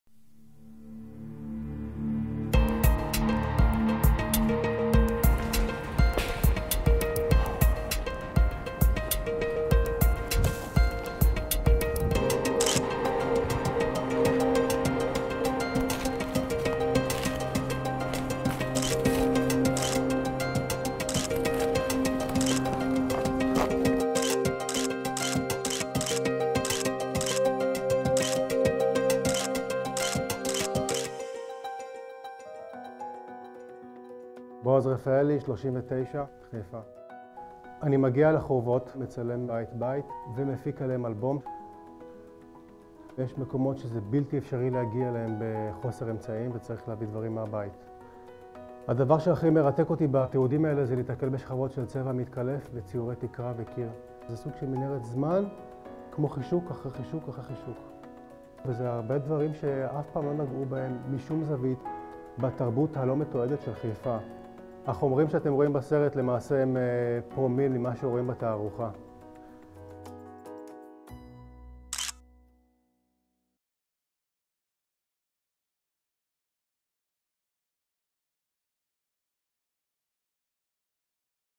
בועז רפאלי - תערוכת "אדריכלות ערבית: חיפה" (1860-1930)
בועז רפאלי, שחלק מעבודותיו מוצגות בתערוכה "אדריכלות ערבית: חיפה" (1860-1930) במוזיאון העיר חיפה, מספר ומראה לכולם כיצד נוצרים הצילומים שלו ומה בדיוק הוא עושה בשביל לתעד חלק מהמבנים המיוחדים שיש לנו בחיפה.
נעילת התערוכה - 29.05.16